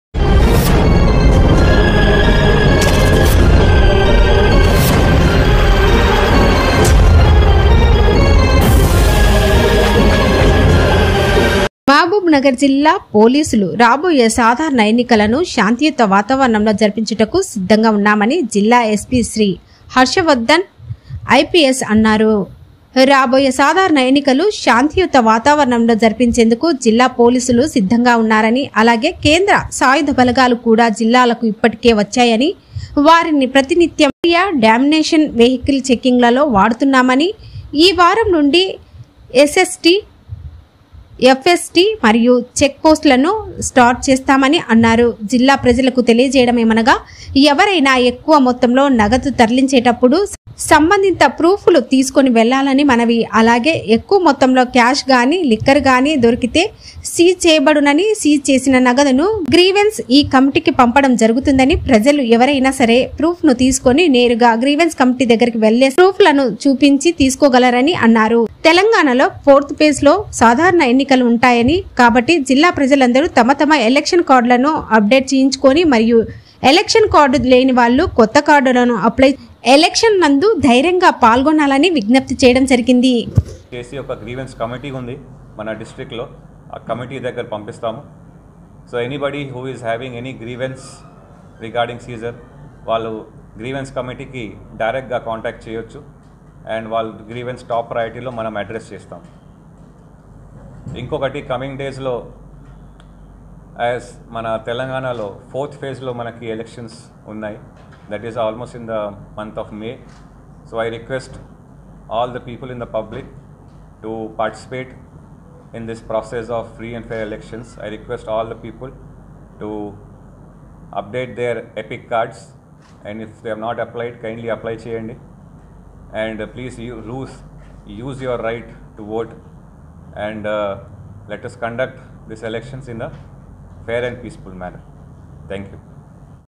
మహబూబ్నగర్ జిల్లా పోలీసులు రాబోయే సాధారణ ఎన్నికలనూ
మహబూబ్నగర్ జిల్లా పోలీసులు రాబోయే సాధారణ ఎన్నికలనూ శాంతియుత వాతావరణం జరిపించుటకు సిద్ధంగా ఉన్నామని జిల్లా ఎస్పీ శ్రీ హర్షవర్ధన్ ఐపీఎస్ అన్నారు.

రాబోయే సాధారణ ఎన్నికలు శాంతియుత వాతావరణంలో జరిపించేందుకు జిల్లా పోలీసులు సిద్ధంగా ఉన్నారని అలాగే కేంద్ర సాయుధబలగాలు కూడా జిల్లాకు ఇప్పటికే వచ్చాయని వారిని ప్రతినిత్యం ఏరియా డామినేషన్ వెహికల్ చెకింగ్ లలో వాడుతున్నామని, ఈ వారం నుండి ఎస్.ఎస్.టి,  ఎఫ్.ఎస్.టి మరియు చెక్ పోస్టులను స్టార్ట్ చేస్తామని అన్నారు, జిల్లా ప్రజలకు తెలియజేయడమేమనగా ఎవరైనా ఎక్కువ మొత్తంలో నగదు తరలించేటప్పుడు సంబంధిత ప్రూఫ్ లను తీసుకొని వెళ్లగలరని మనవి. అలాగే ఎక్కువ మొత్తంలో క్యాష్ గాని లిక్కర్ గానీ దొరికితే సీజ్ చేయబడునని సీజ్ చేసిన నగదును గ్రీవెన్స్ ఈ కమిటీకి పంపడం జరుగుతుందని, ప్రజలు ఎవరైనా సరైన ప్రూఫ్ ను తీసుకొని నేరుగా గ్రీవెన్స్ కమిటీ దగ్గరికి వెళ్లి సంబంధిత ప్రూఫ్లను చూపించి తీసుకోగలరని అన్నారు.
తెలంగాణలో ఫోర్త్ ఫేస్ లో సాధారణ ఎన్నికలు ఉంటాయి కాబట్టి జిల్లా ప్రజలందరూ తమ తమ ఎలక్షన్ కార్డులను అప్డేట్ చేసుకొని మరియు ఎలక్షన్ కార్డు లేని వాళ్ళు కొత్త కార్డులను అప్లై చేసుకుని ఎలక్షన్ నందు ధైర్యంగా పాల్గొనాలని విజ్ఞప్తి చేయడం జరిగింది.